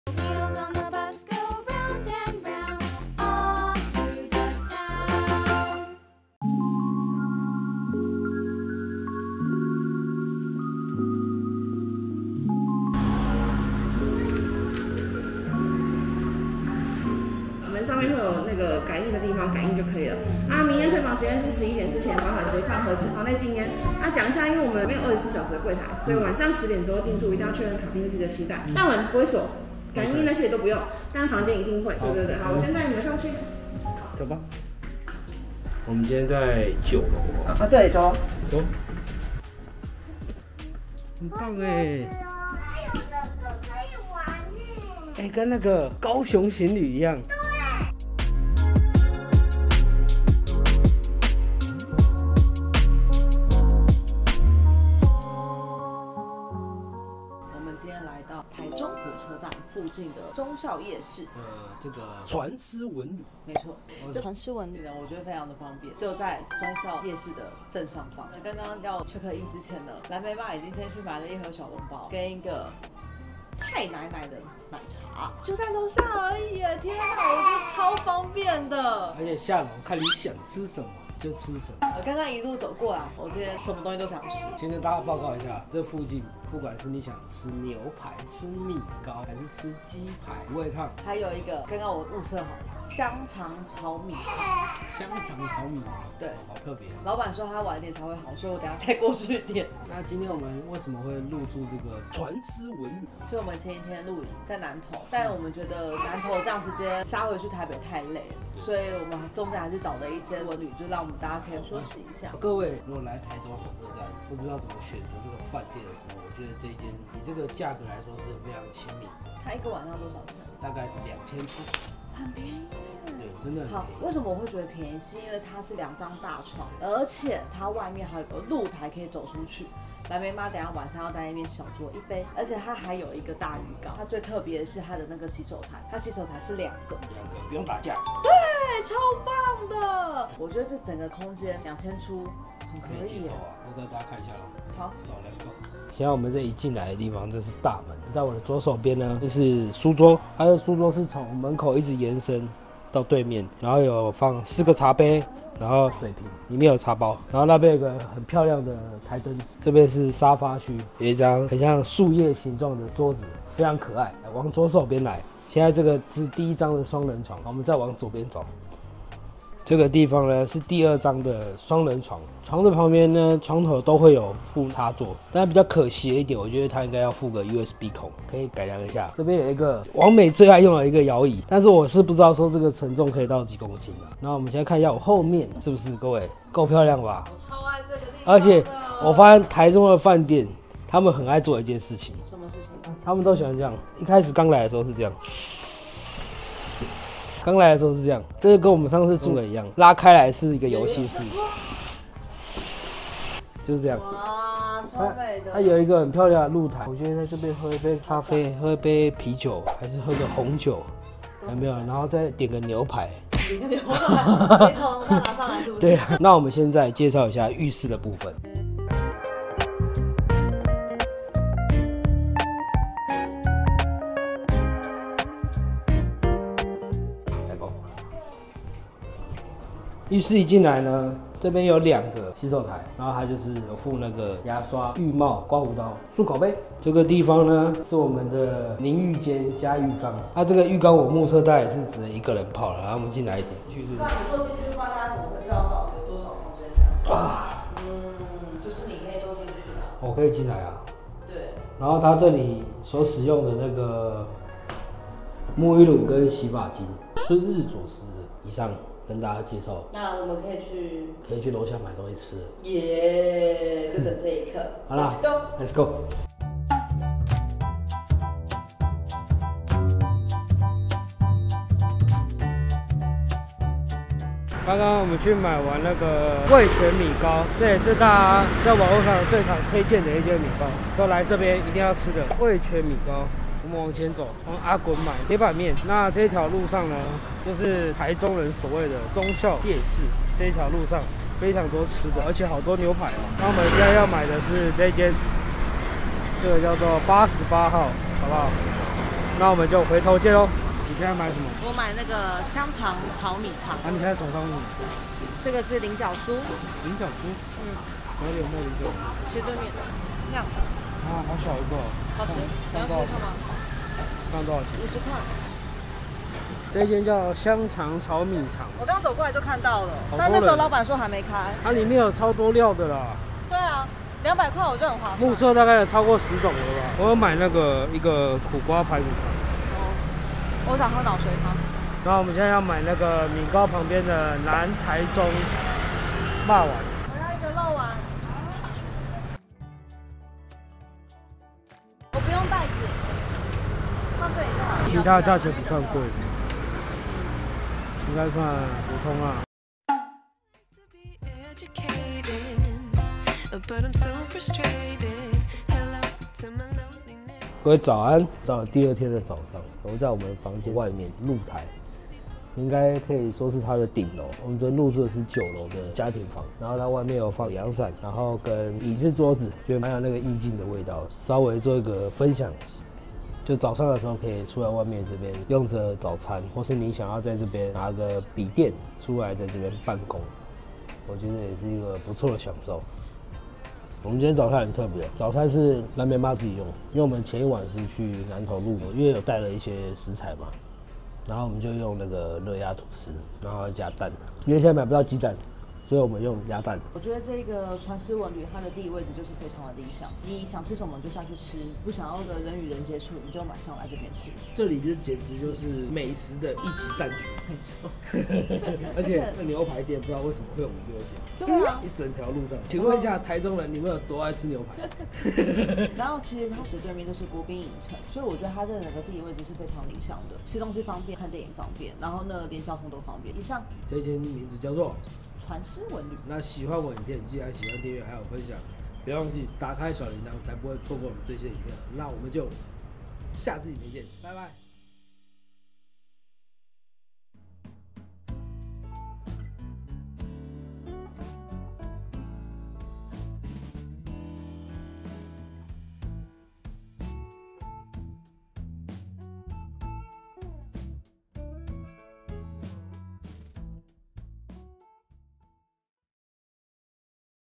【超浮誇房型】窗簾拉開竟然是座空中花園｜Trans Inn傳思文旅｜陳儒爸與藍莓媽
前晚在南投露營後~我們來到忠孝夜市
這裡的傳斯文旅地理位置超理想💯

自從有了孩子後,逛夜市已經不再是隨心所欲的享受
我們總是要考慮哪些食物適合孩子們
🚫竹籤 🚫湯湯水水 🚫辣的 🚫容易掉的
相信許多父母都和我們一樣,為孩子的安全心中無限小劇場🤦‍♀️

不過旅店就在夜市裡讓我們可以放下顧慮
開心買 瘋狂買 到處買 通通買回飯店享用😎
味全米糕、炒雙腸、傳統烤魷魚、麻辣臭豆腐、菱角酥、
鐵板牛排、鮮奶甘蔗汁、麵線糊、新疆口肉串、冷盤滷味、檸檬甜甜圈….((到底是多餓🤣
這時候多希望大胃王上身,把這邊的所有美食全吃遍

夜晚還沒有結束,我們回到房間的露臺上
享受著小酌一杯的寧靜時光🌙
住在這樣方便又舒適的地方,讓我們度過了一個非常愉快的旅行
🎬熱門影片播放清單👇
拍攝器材⬇︎
DJI Pocket 2
IPhone 8
剪輯工具⬇︎
Adobe Premiere Pro